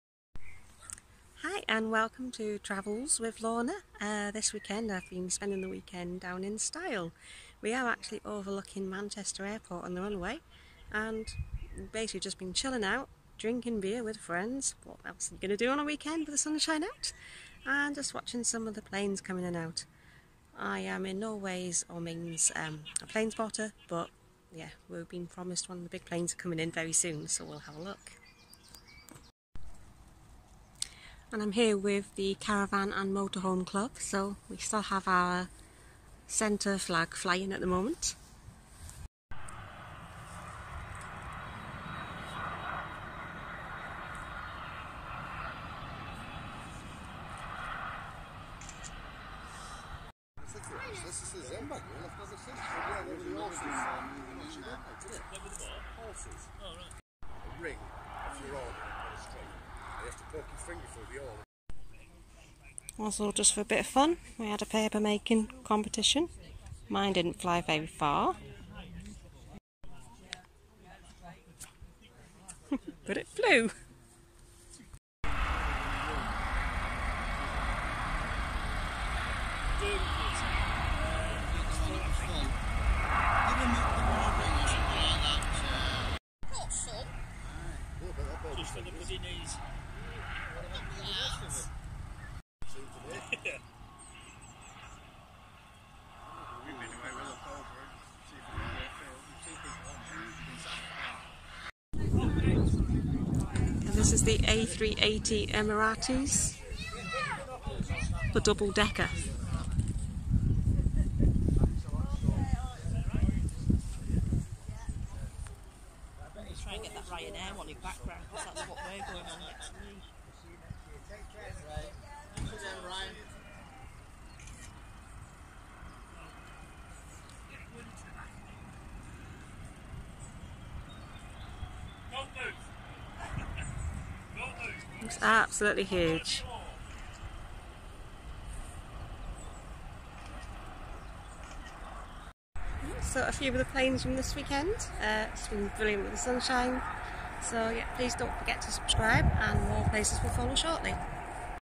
Styal rally overlooking Manchester airport featuring emirates airbus A380 double decker
A weekend away at caravan and motorhome club rally field in Styal, alongside Manchester airport.

The planes were busy all weekend and a few of them captured on film